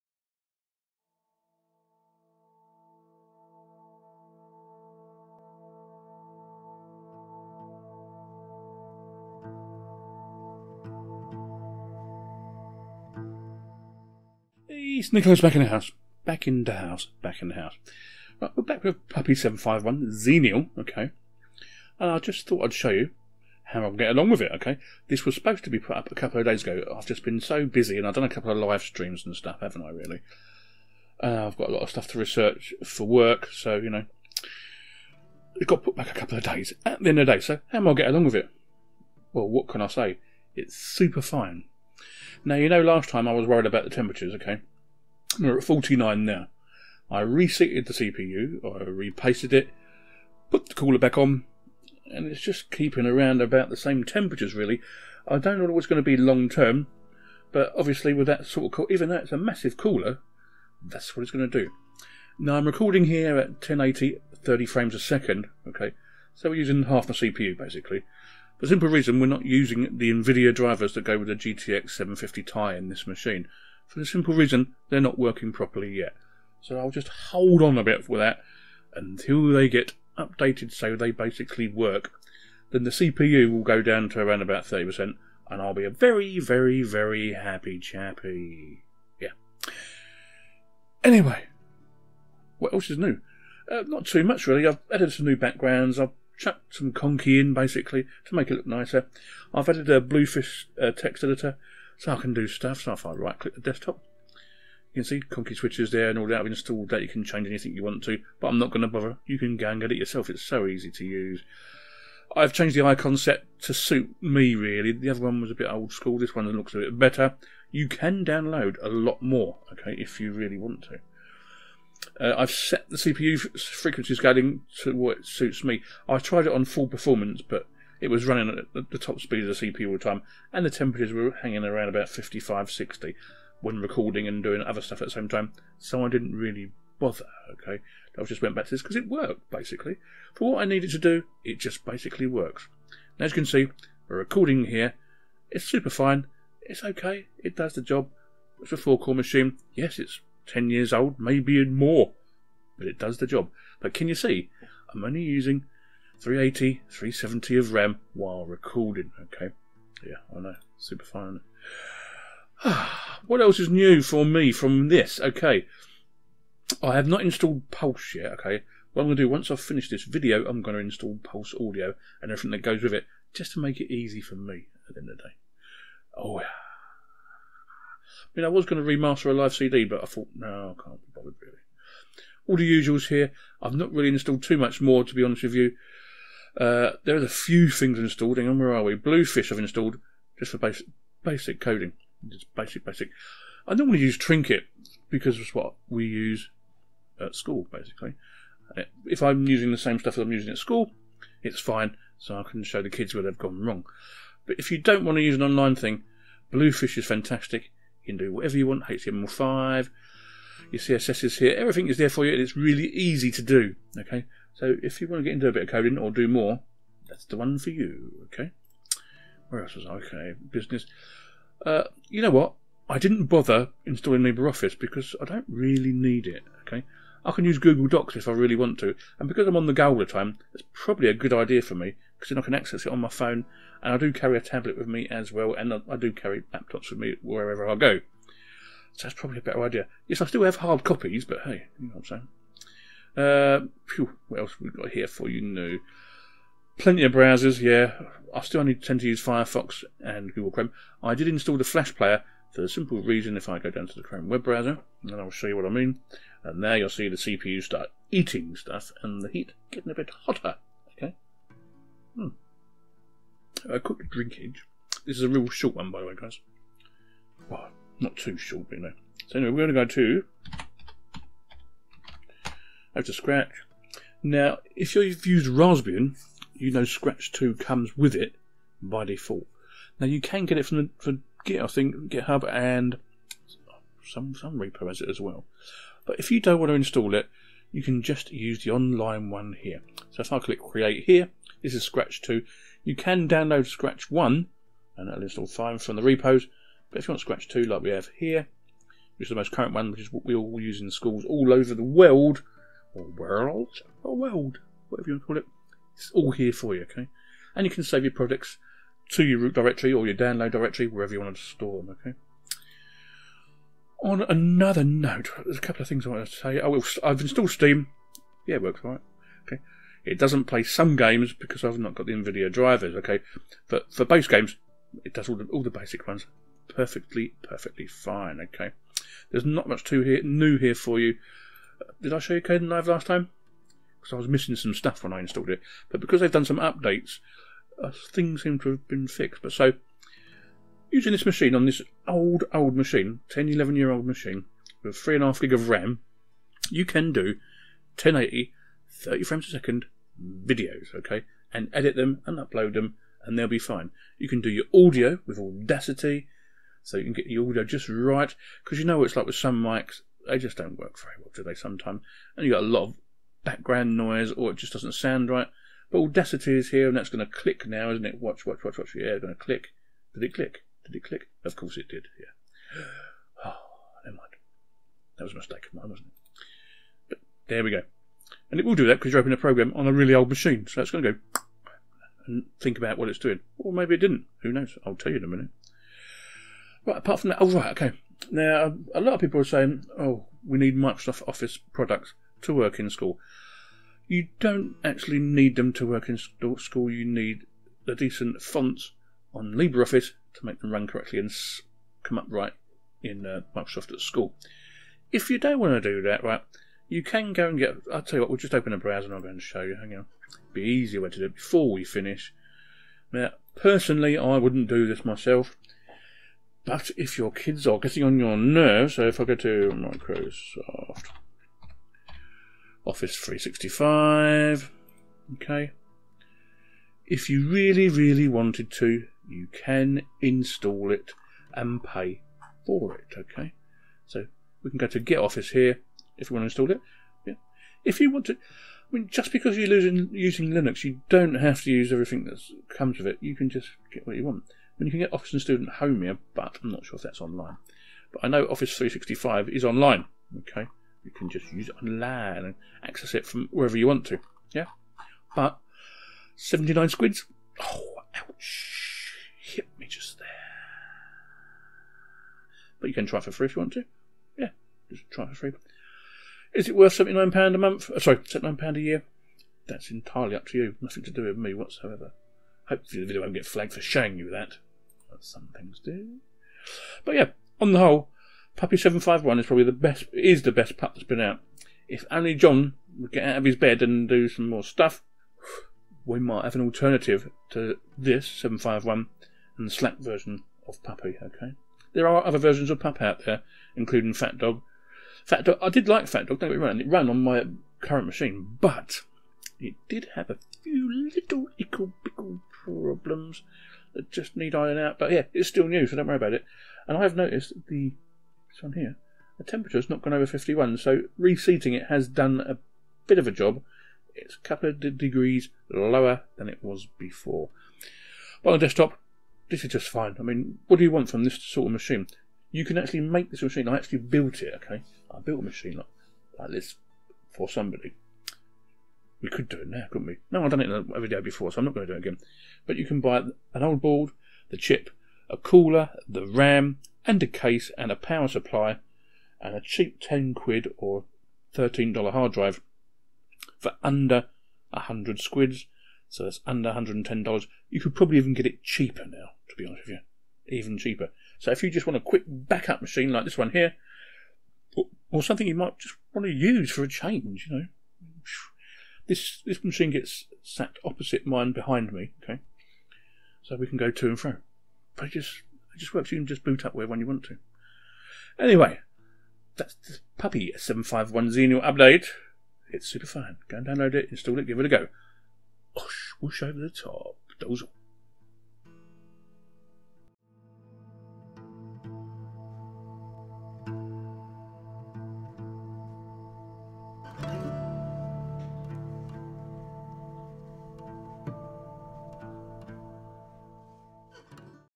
Puppy Linux 751 Update.....It's Going Real Fine...
Here is just an update on how i am getting along with this latest version of Puppy Linux and all is good and everything just works fine and considering this machine is now 10-11 years old you cant really knock it.
So overall if you have limited hardware and the latest and greatest are not doing it for you go download a puppy Linux distribution as you will not regret it...nice..